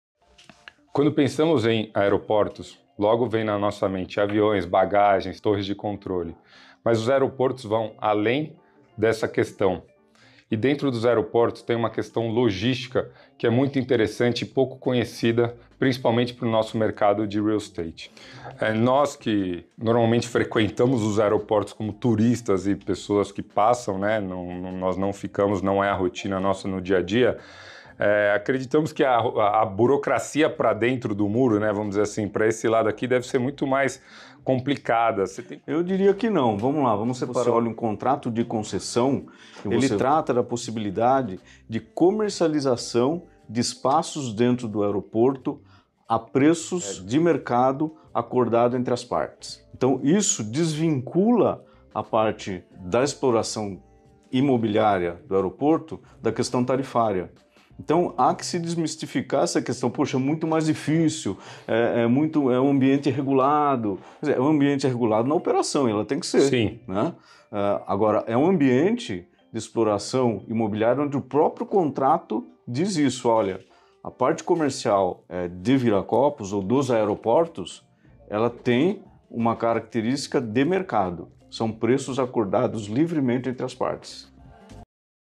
Podcast | Desenvolvimento dentro de aeroportos é possível? Cesar Worms, gerente em Viracopos comenta
🚀 Quer entender o mundo dos aeroportos além dos voos e malas? 🛫✈ Dê uma espiada nos bastidores com a SiiLA! Descubra o potencial para o desenvolvimento de imóveis logísticos dentro dos aeroportos🔍
Esta é uma entrevista exclusiva do nosso projeto SiiLAPODCAST, onde conversamos com Cesar Worms, gerente de negócios imobiliários de Viracopos. Confira o conteúdo completo nos nossos canais de media da SiiLA e no